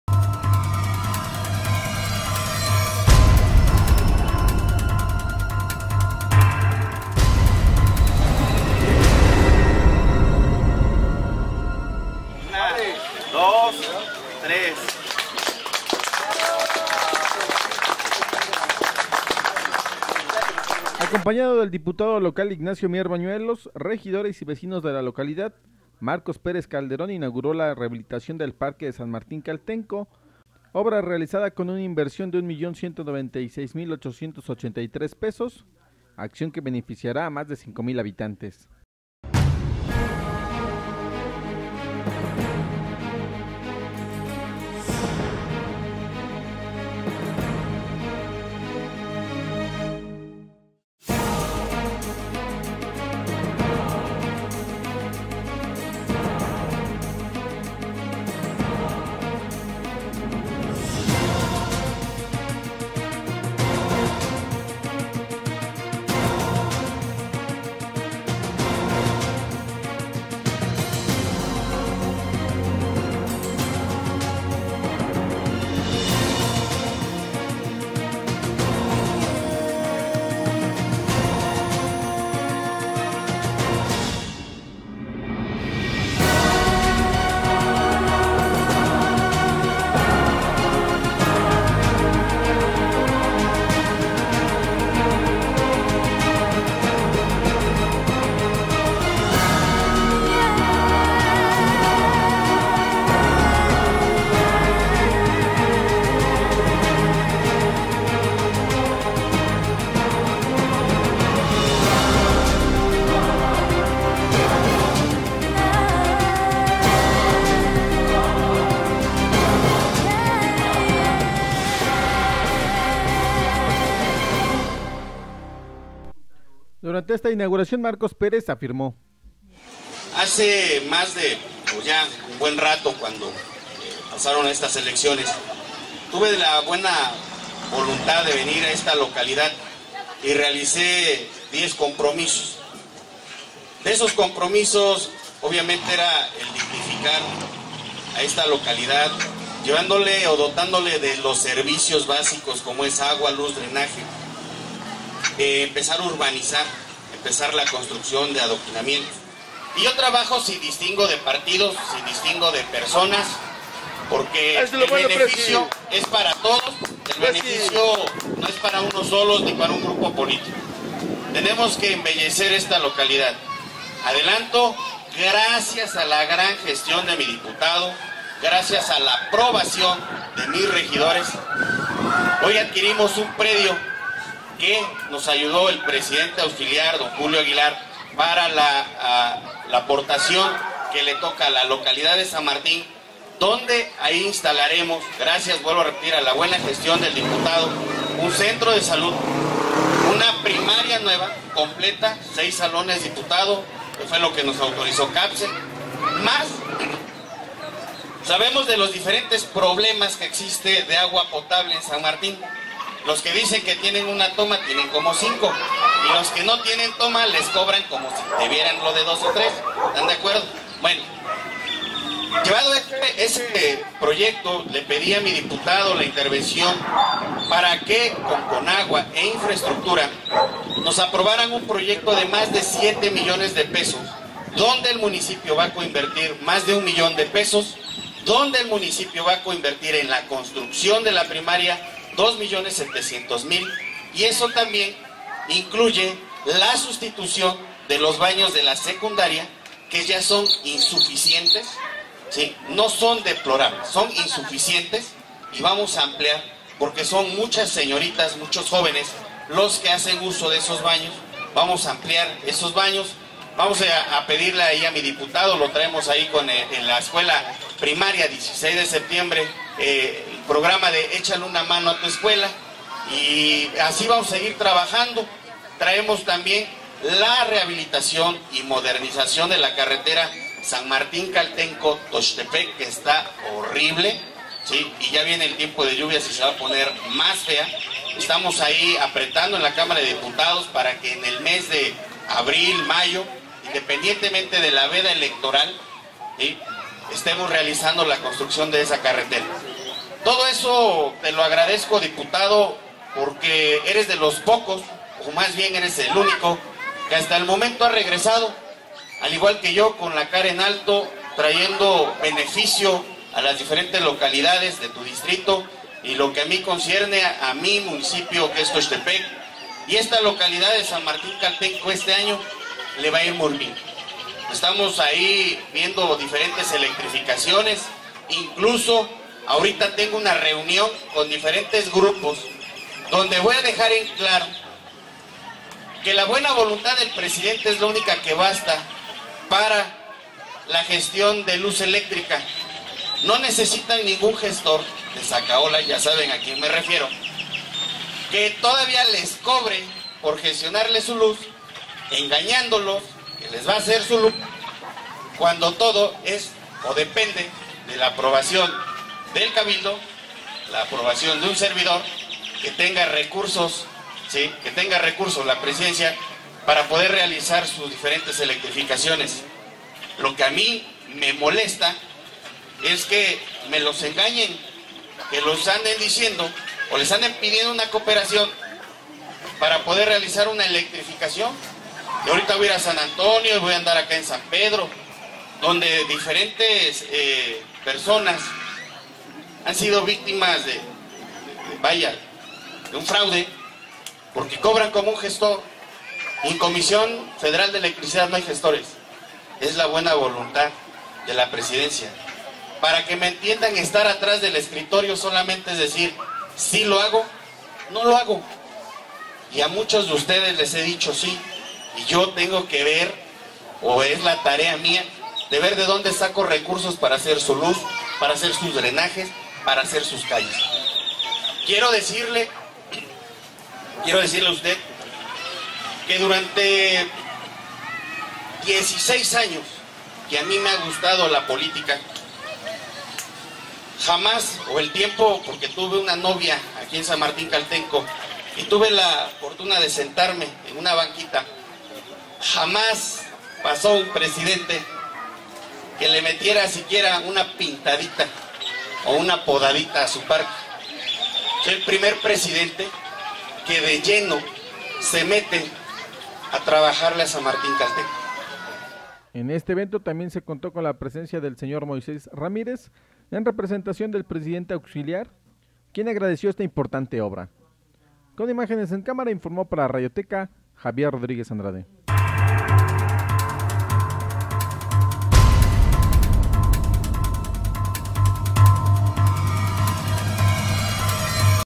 Parque de San Martín Caltenco.
El Alcalde de Tochtepec, Marcos Pérez Calderón inauguró la Rehabilitación del Parque de la Junta Auxiliar de San Martín Caltenco.